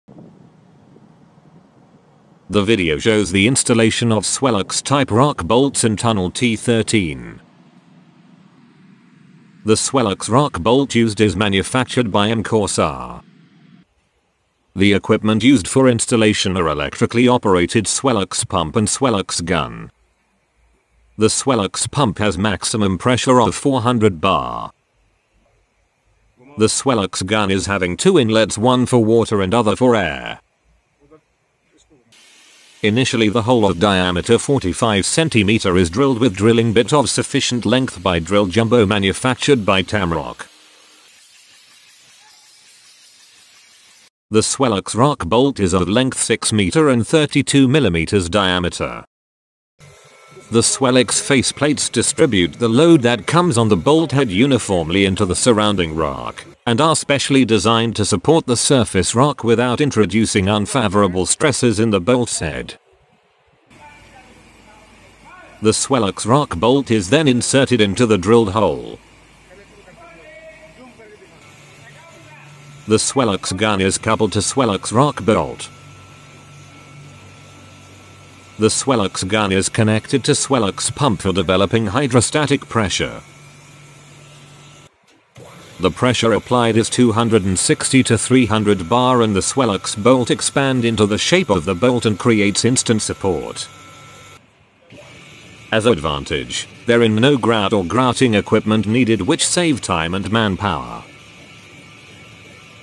Swellex Rock bolt installation in tunnels
The video shows the installation of Swellex Type rockbolt in tunnels.